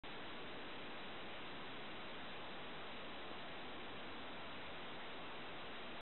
6 Second Videos - Three Slugs Eat One Dead Slug
Six Second Videos - Three Slugs Eat One Dead Slug -  Two bigger slugs and one tiny slug (harder to see) are eating the dead slug on the sidewalk tonight. Why is this happening??